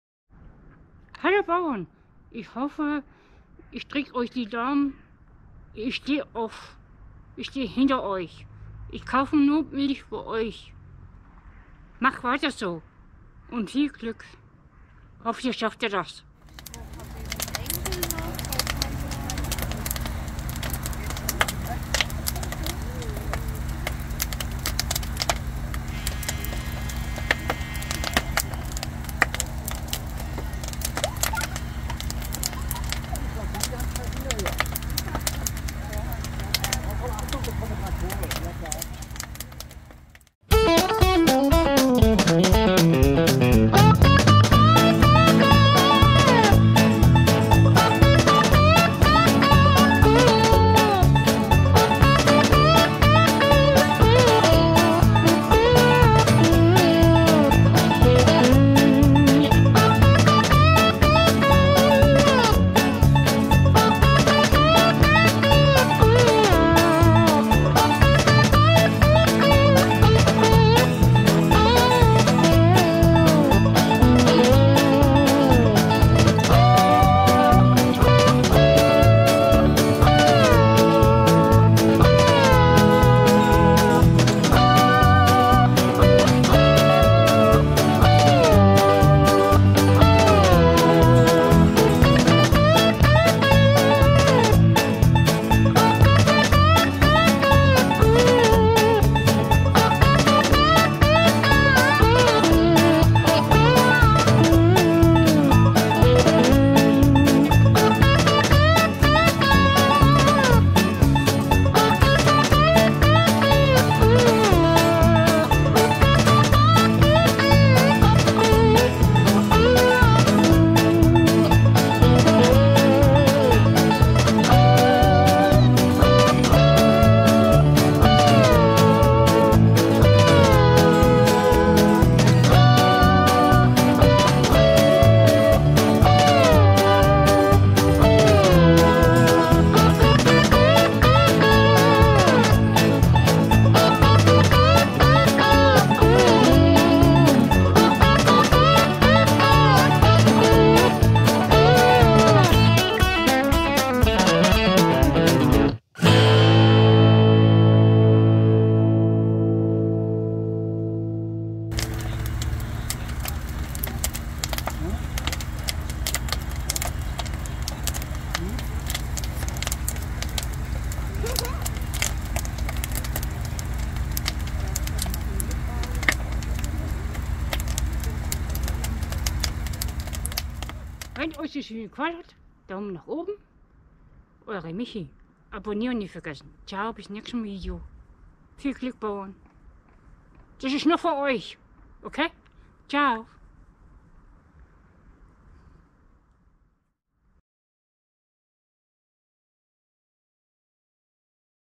Mahnfeuer gegen die Pläne der Regierung
Hallo Freunde und Bauern
Mit Diesen Video möchte ich euch zeigen ich halte zu euch ! Und gegen unsre Mise Ampel Regierung
Ich wünsche euch viel Erfolg bei euren Protest und weg Mit der Ampel kämpft für euer Recht .
Ich stehe heiter euch !
Viel Spaß auf diesen Video
Grüße Michi